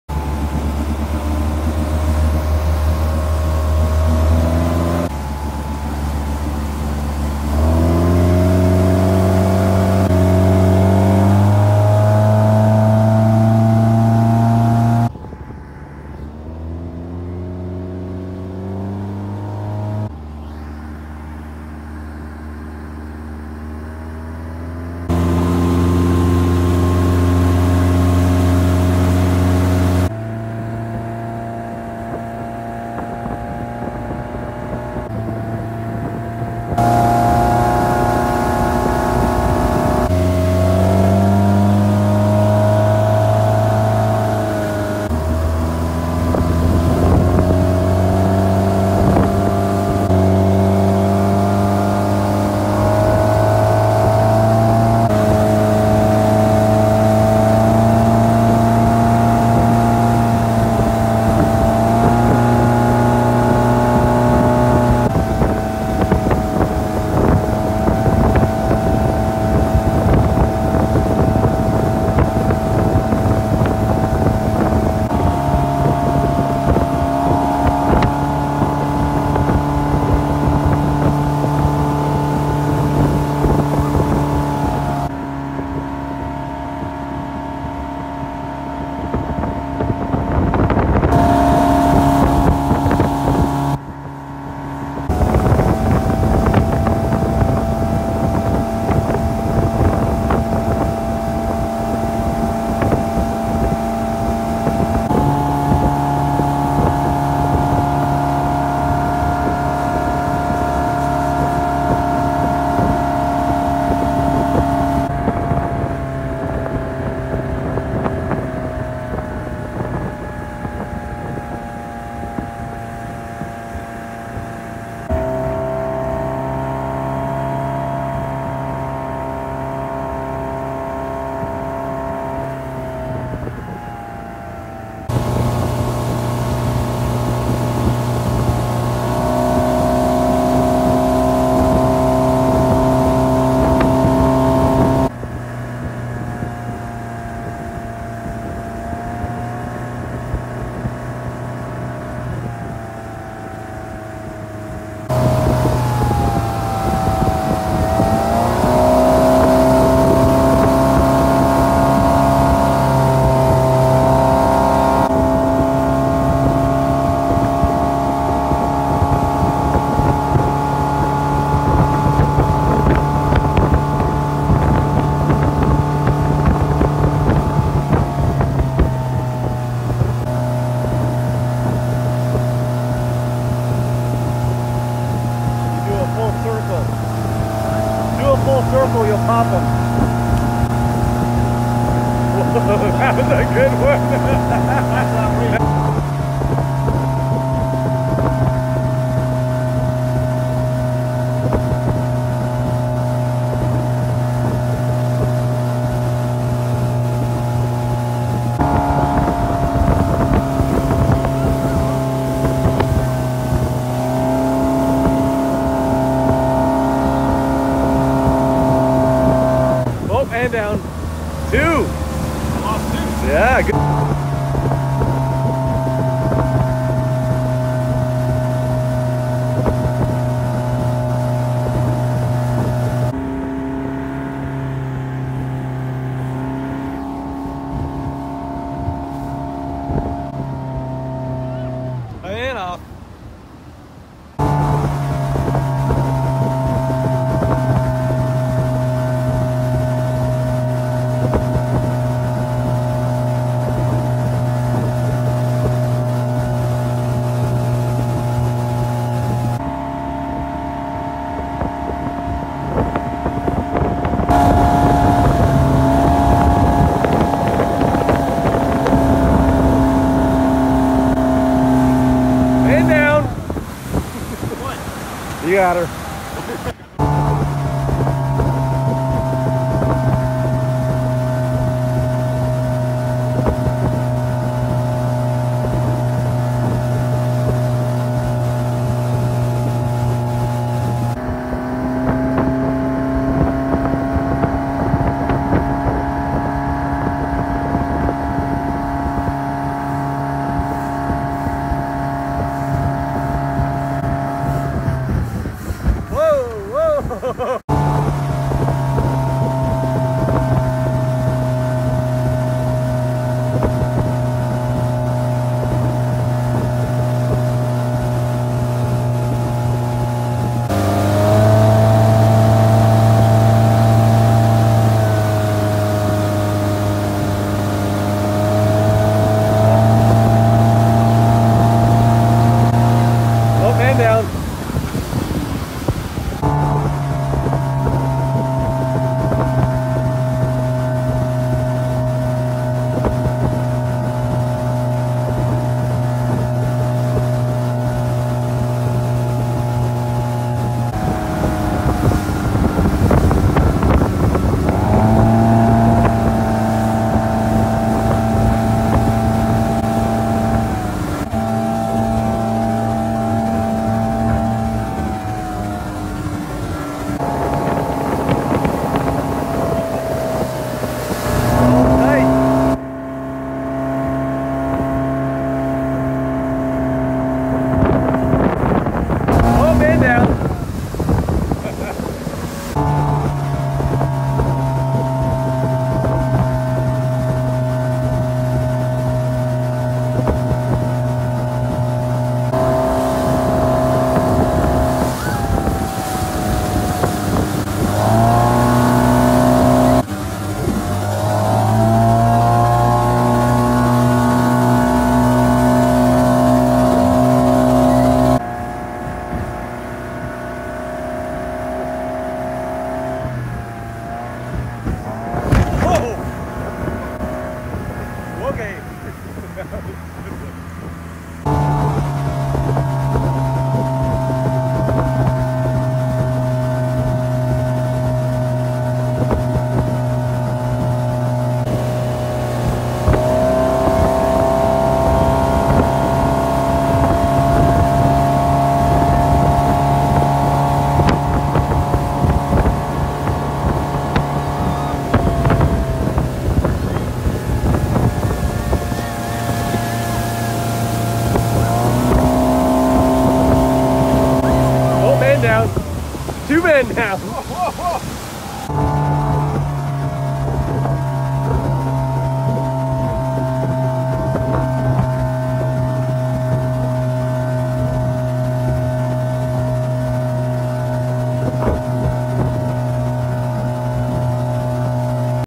Clear Lake Tubing Supercut
Lots of people tubing at Clear lake with wipe outs at the end. No music so you can make out the screams of joy/terror